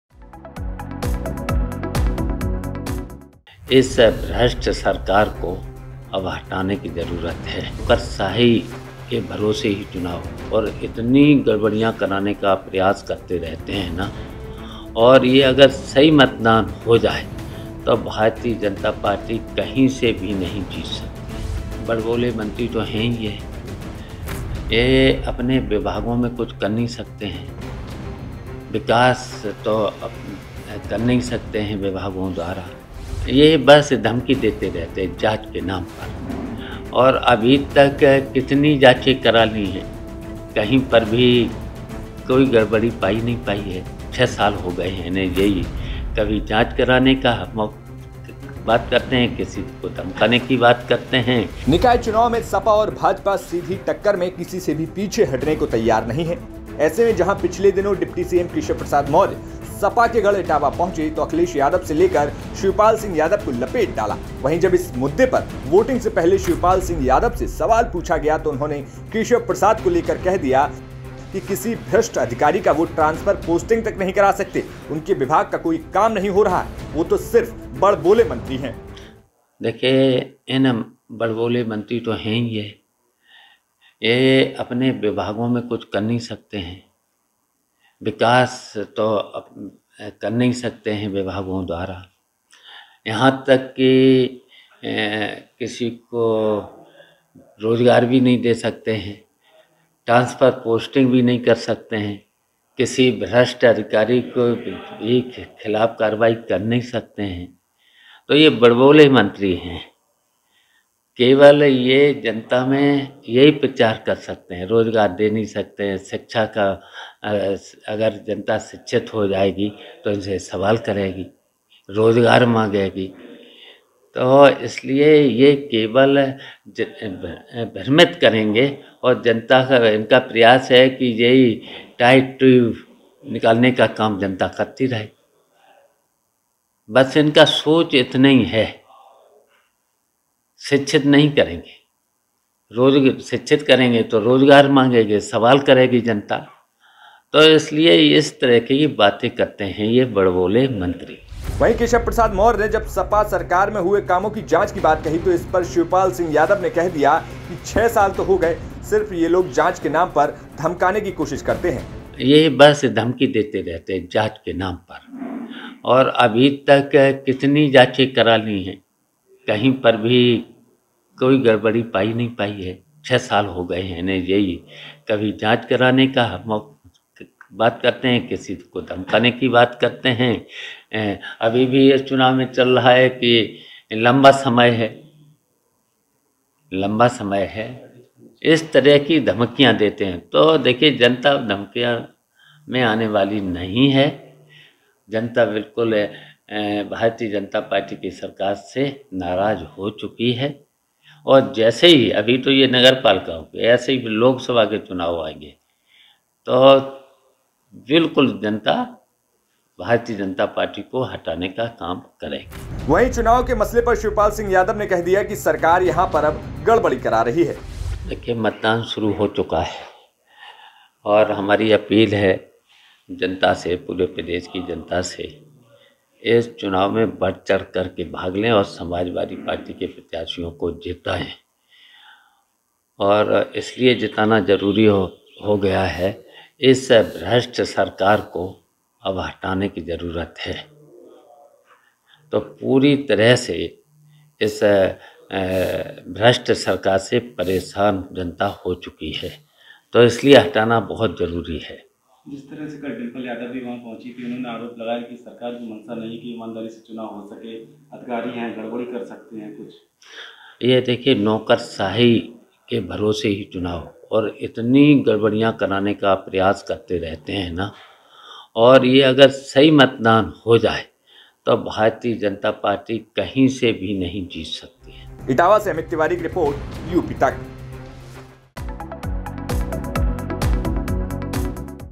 Shivpal Singh Yadav : डिप्टी सीएम केशव प्रसाद मौर्य पर फायर हो गए शिवपाल कहा..
Shivpal Singh Yadav : निकाय चुनाव में सपा और भाजपा सीधी टक्कर में किसी से भी पीछे हटने को तैयार नहीं है ऐसे में जहां पिछले दिनों डिप्टी सीएम केशव प्रसाद सपा के गढ़ इटावा पहुंचे तो अखिलेश से लेकर शिवपाल को लपेट डाला. वहीं जब इस मुद्दे पर वोटिंग से पहले शिवपाल सिंह यादव से सवाल पूछा गया तो उन्होंने केशव प्रसाद को लेकर कह दिया कि किसी भ्रष्ट अधिकारी की वो ट्रांसफर पोस्टिंग करा नहीं सकते, उनके विभाग का कोई काम नहीं हो रहा, वे तो सिर्फ बड़बोड़े मंत्री हैं.